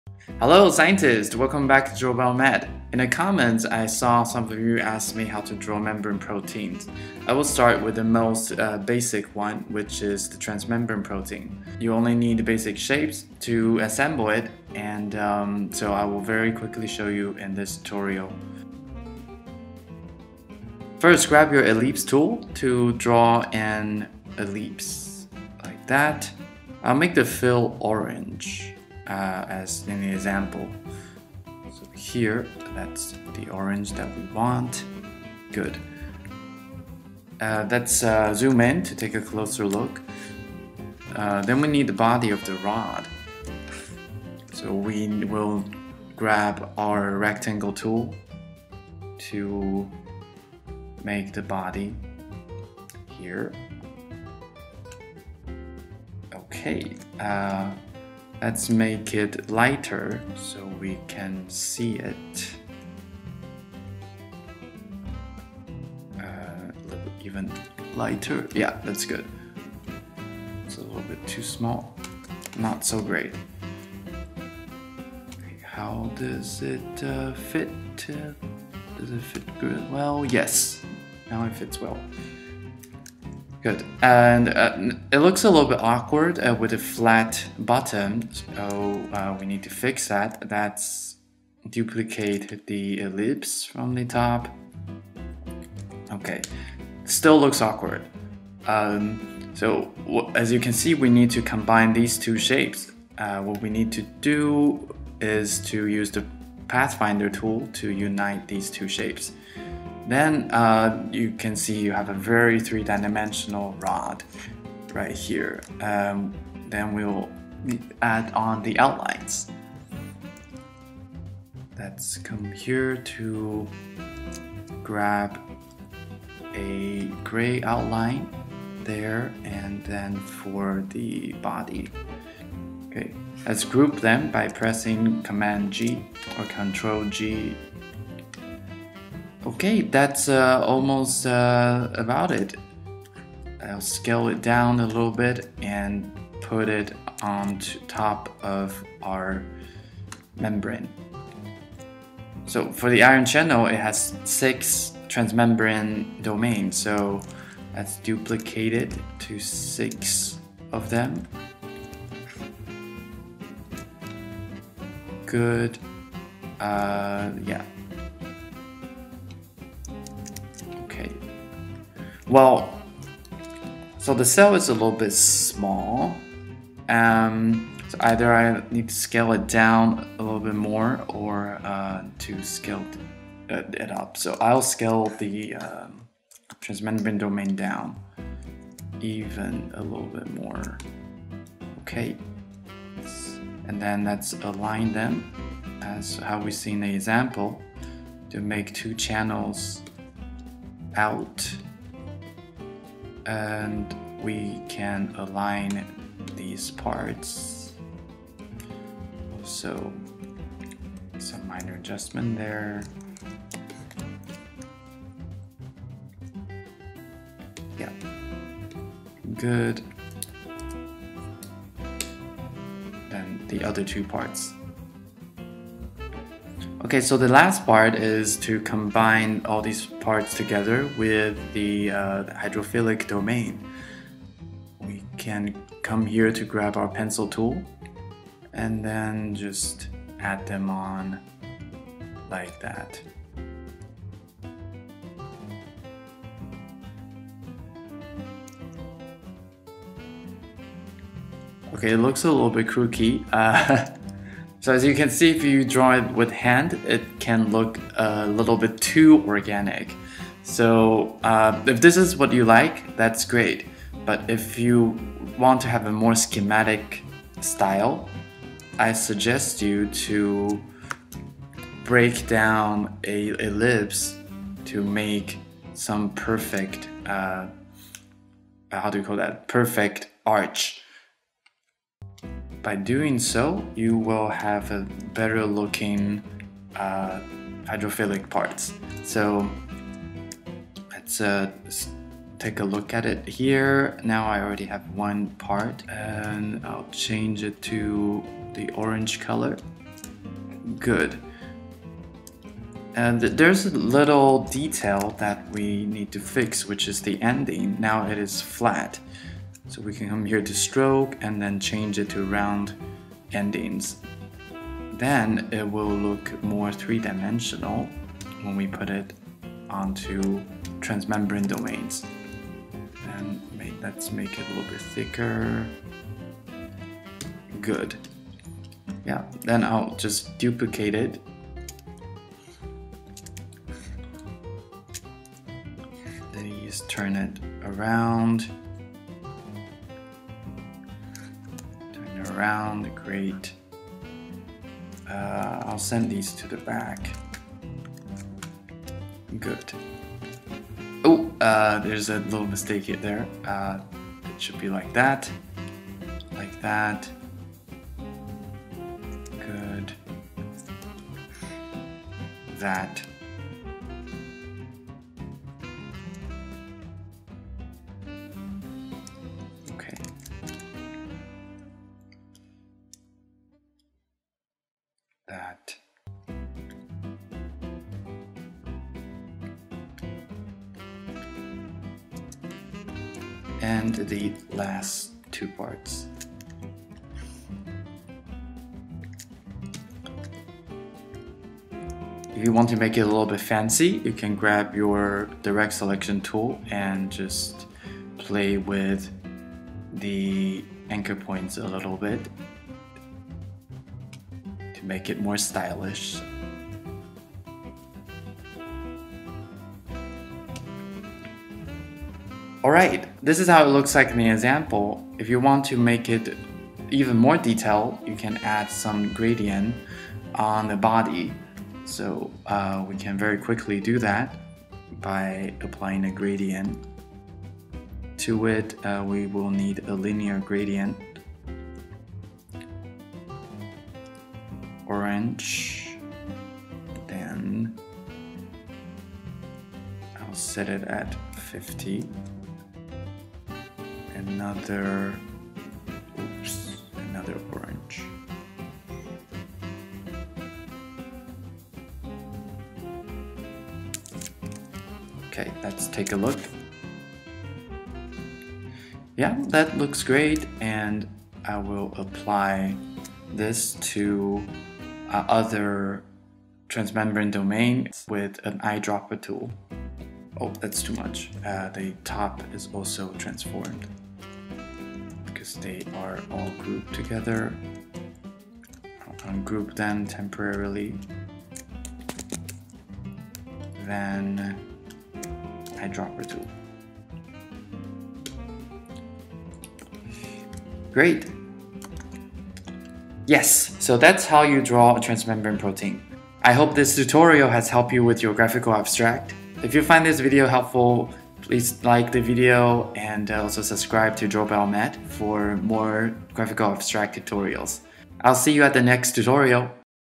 Draw transmembrane protein in Adobe illustrator | Illustrator for scientists | Graphical abstract
This tutorial shows scientist how to draw transmembrane proteins in Adobe illustrator, so scientists like you can make professional scientific illustration for your research publication, conference posters, science figures and graphical abstracts.

Style: Nature publication graphics & figures.

🎨 DrawBioMed is a channel for scientists to learn professional scientific illustrations for their research.

Technique: Expend

Nature review example:
The lysosome as a cellular centre for signalling, metabolism and quality control